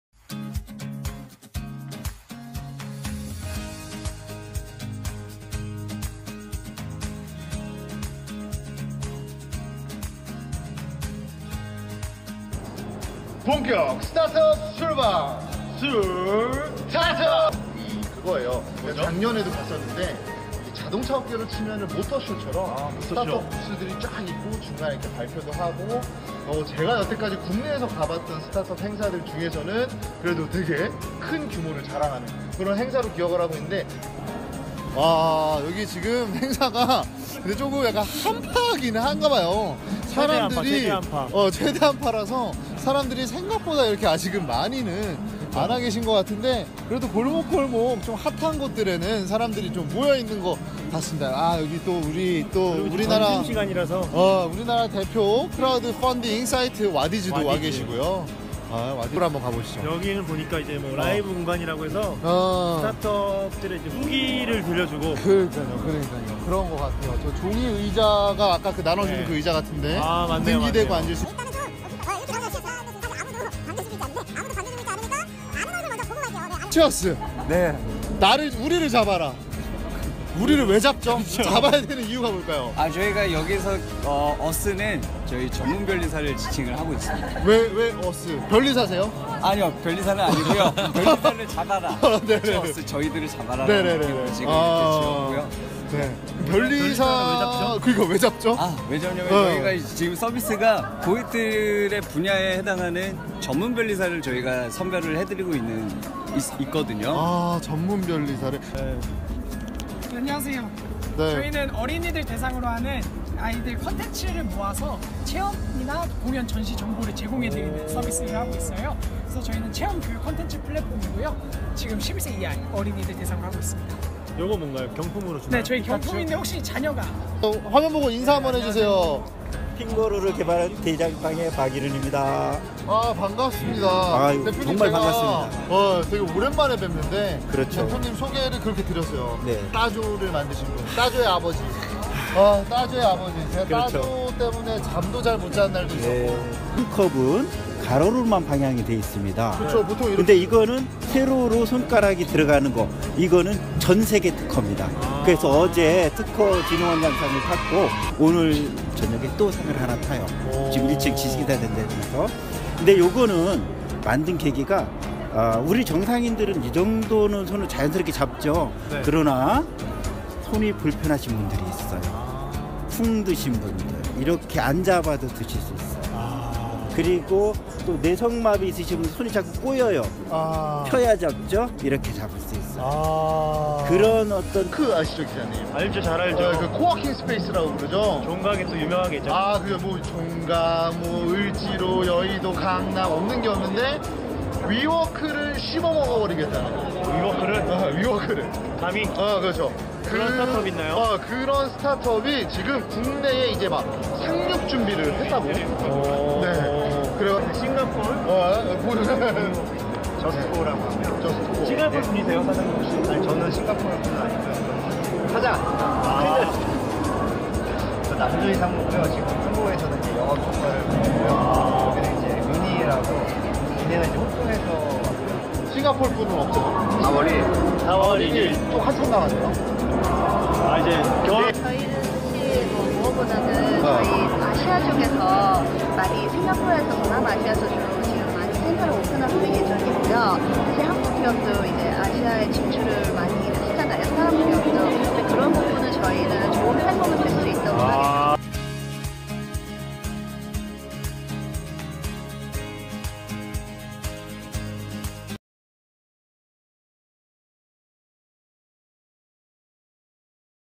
[술타트업] 장애인을 위한 컵 '핑거루', 사무실 공유업체 '저스트코'를 만나다
술타트업은 스타트업 종사자와 만나 진솔하게 술을 한 잔하면서 이야기를 나누는 코너입니다.  스타트업 분야가 재밌는 소재가 아닌만큼 사람사는 이야기를 위주로 다루려고 합니다. 스타트업을 준비 중인 분, 스타트업을 종사하시는 분들이 재밌게 봐주셨으면 합니다.

이번 영상에서는 지난 8일부터 9일까지 코엑스에서 진행한 헤이스타트업 현장을 다녀왔습니다. 현장에 수많은 스타트업 관계자들이 있었는데요, 그분들을 만나서 이야기를 들었습니다.

휴가 관계로 급하게 편집하느라 다음영상에서 미쳐 담지 못한 인터뷰도 함께 올려드리겠습니다.